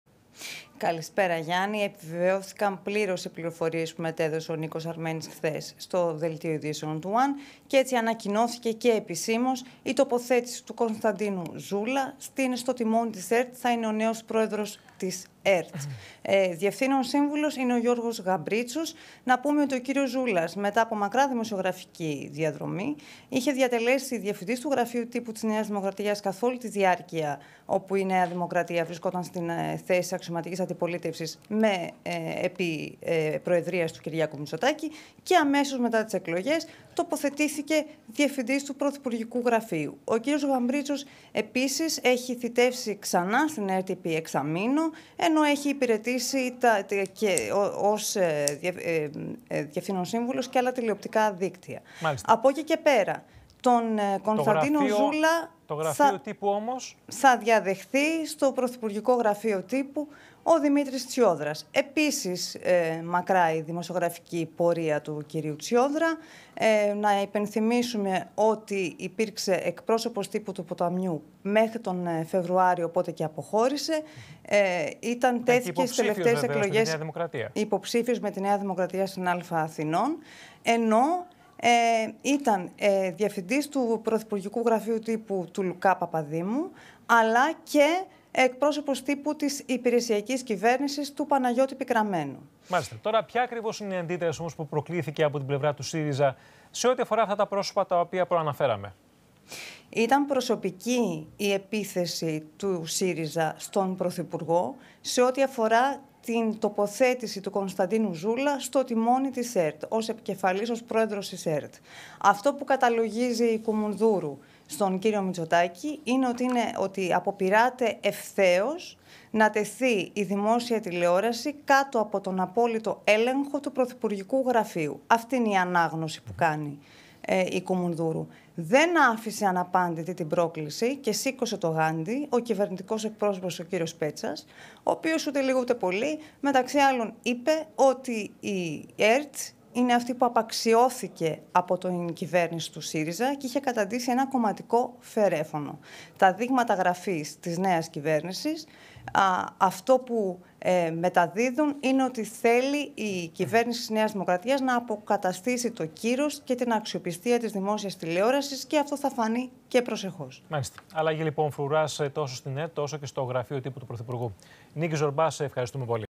Πρόεδρος στην ΕΡΤ ο Κ. Ζούλας - Ο Δ. Τσιόδρας νέος Διευθυντής στο Γραφείο Τύπου του πρωθυπουργού
Αντιδράσεις από τον ΣΥΡΙΖΑ προκάλεσε η ανακοίνωση των προσώπων για την ΕΡΤ.  Στη θέση του προέδρου της δημόσιας τηλεόρασης τοποθετείται ο Κωνσταντίνος Ζούλας, ενώ παράλληλα το Μέγαρο Μαξίμου ανακοίνωσε και τα ονόματα που θα στελεχώνουν πλέον το γραφείο τύπου του πρωθυπουργού.